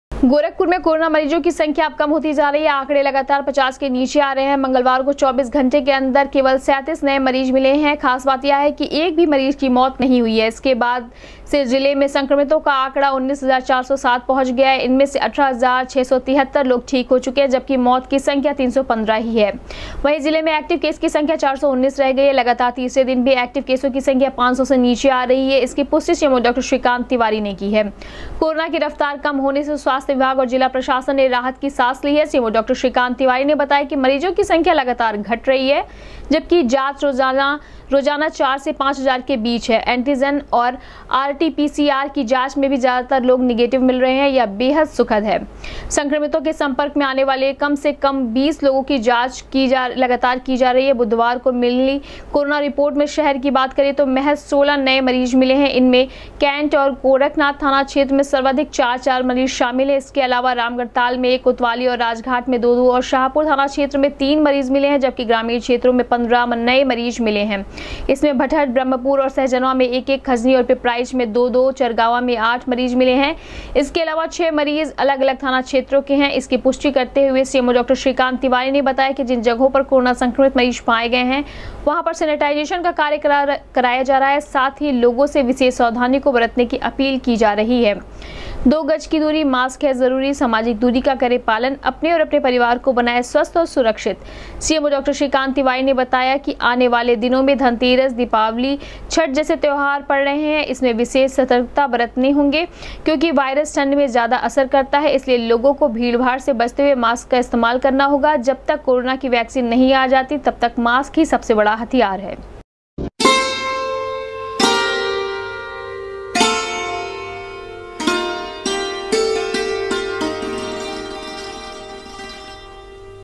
गोरखपुर में बुधवार को मिले 37 नए कोरोना संक्रमित मरीज, जिले में संक्रमित मरीजों की संख्या हुई 19407
गोरखपुर में बुधवार को मिले 37 नए कोरोना संक्रमित मरीज, जिले में संक्रमित मरीजों की संख्या हुई 19407, अब तक 315 कोरोना संक्रमित मरीज गवां चुके हैं अपनी जान, 419 एक्टिव हुए मरीज, सीएमओ डॉ श्रीकांत तिवारी ने की पुष्टि